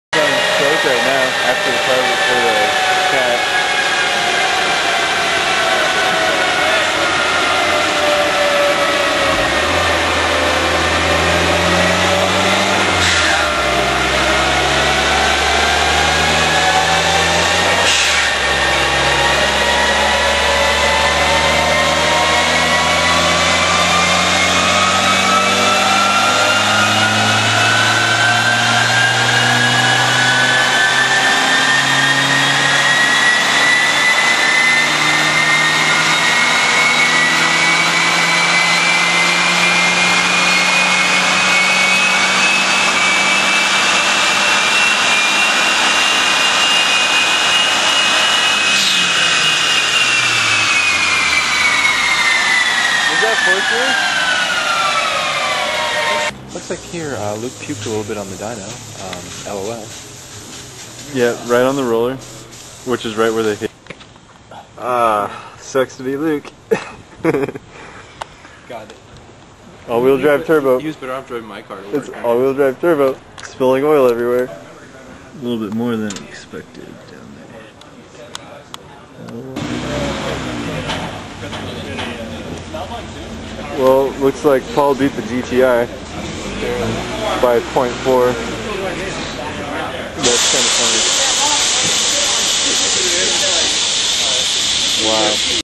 Mazda 323 GTX 1.6L turbo dyno run AWD
88 Mazda 323 GTX (turbo awd) 145whp @ 5900 rpm, 146 trq @ 3300 rpm running 15psi, but only 12.7psi at peak hp, then spilled a little oil on the dyno. This was done on a  Mustang Dyno